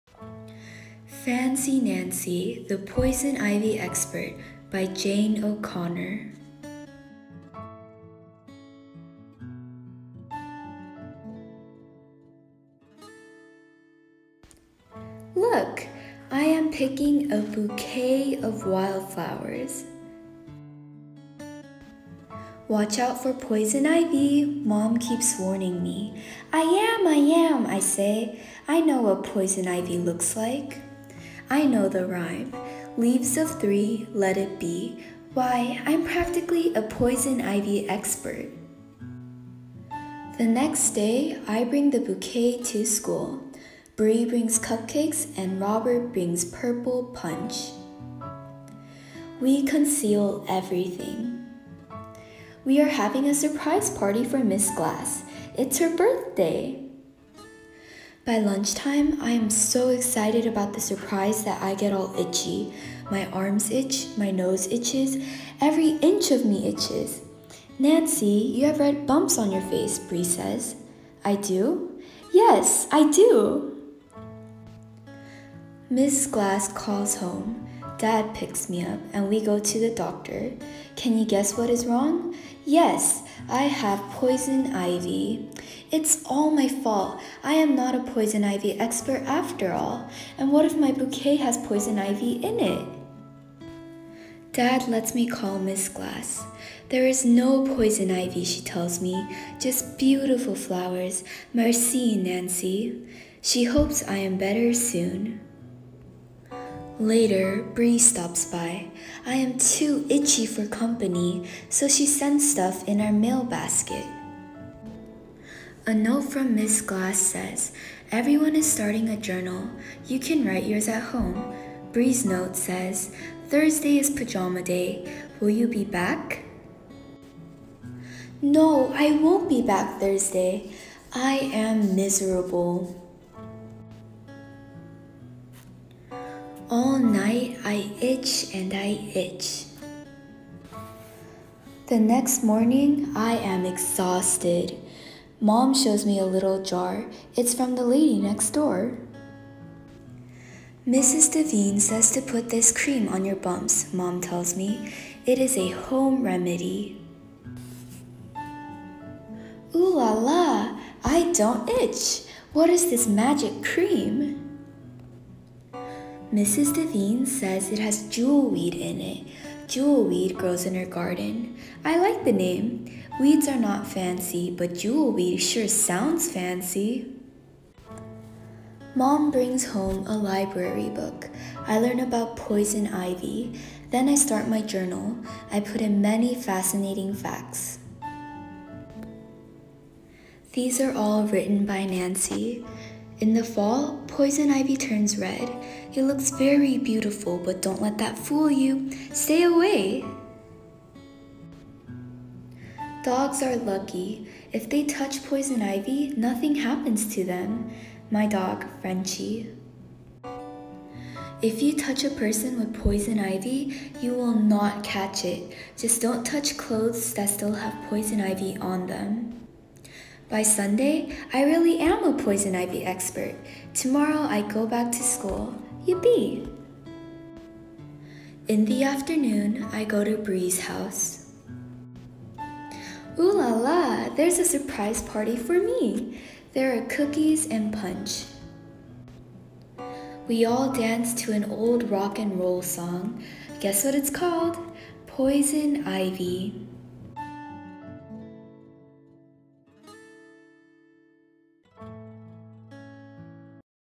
Fancy Nancy: POISON IVY EXPERT!!
(vocabulary + definitions are provided)
~ recommended for preschool and above ~
author: Jane O’Connor

Nancy thinks she is a poison ivy expert. However, after getting infected from the ivy, she realizes she has a lot to learn!